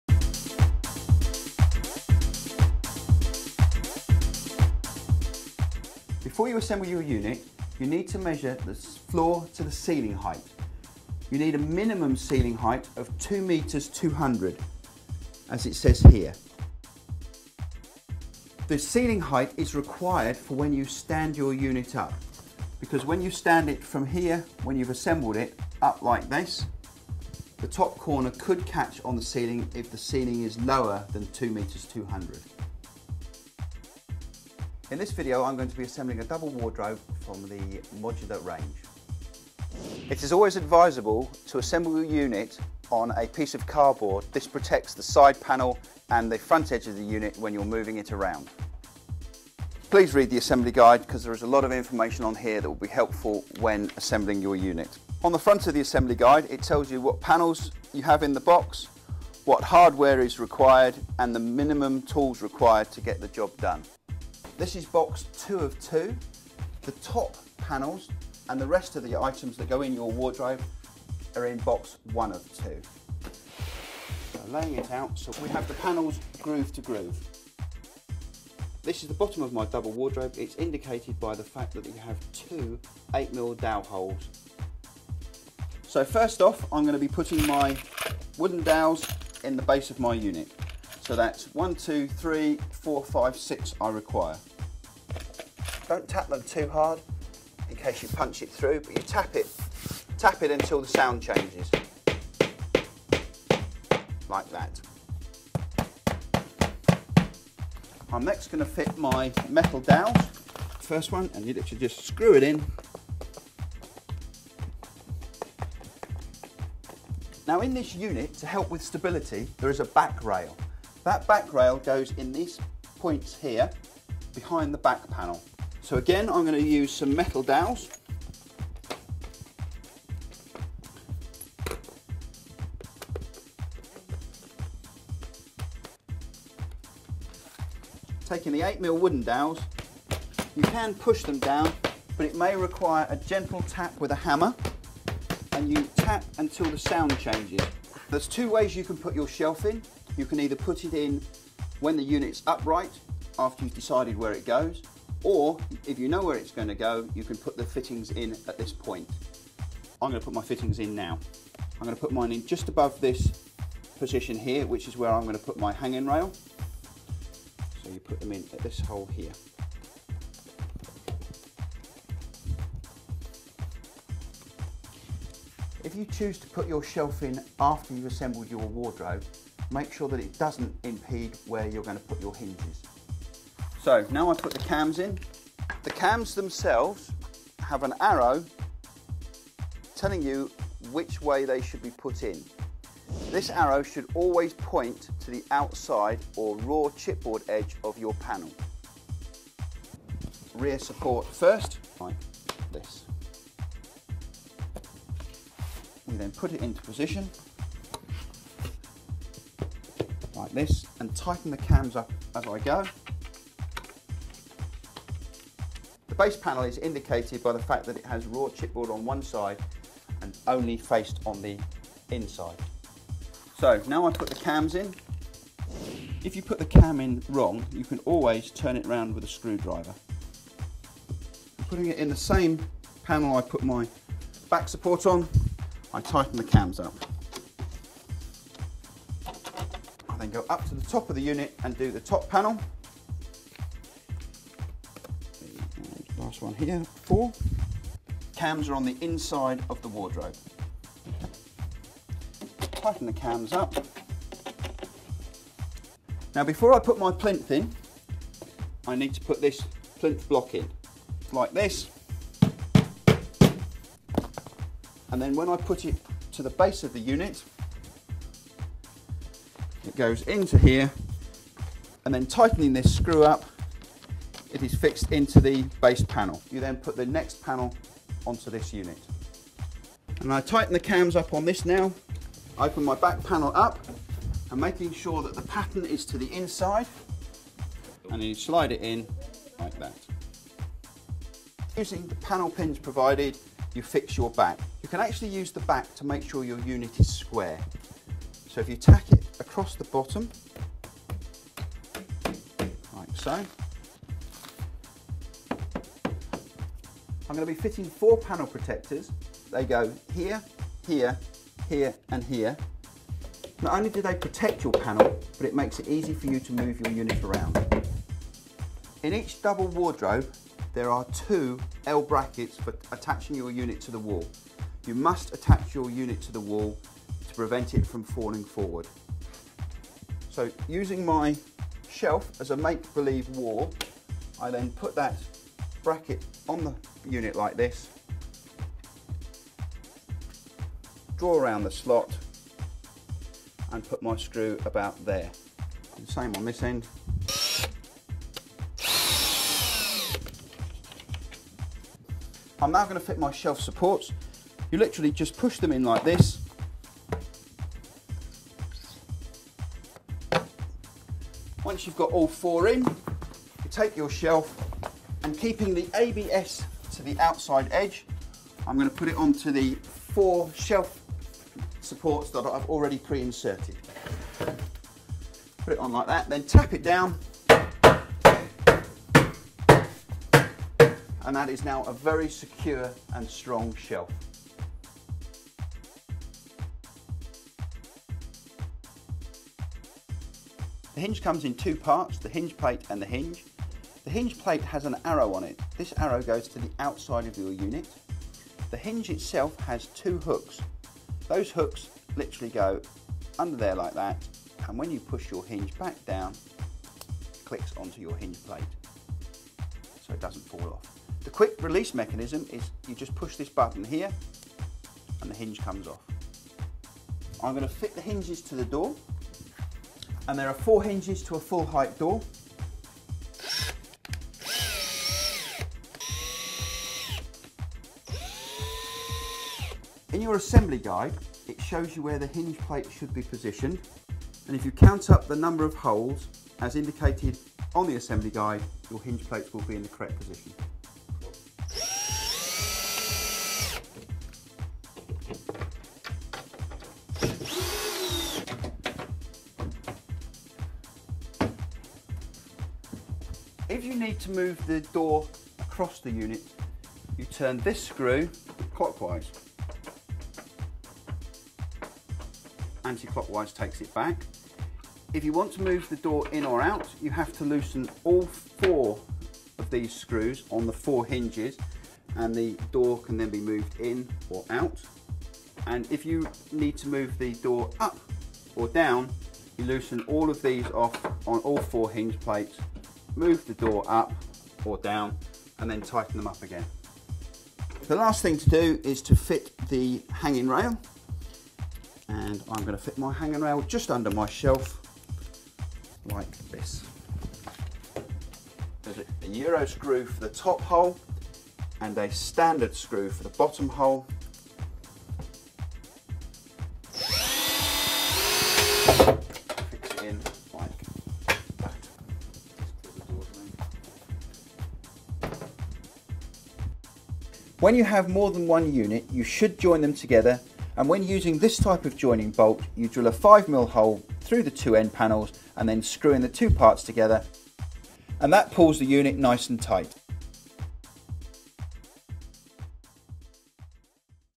Modular Range - Assembling a Double Wardrobe
A step-by-step guide on how to assemble a Double Wardrobe in the Modular range, by Norbord.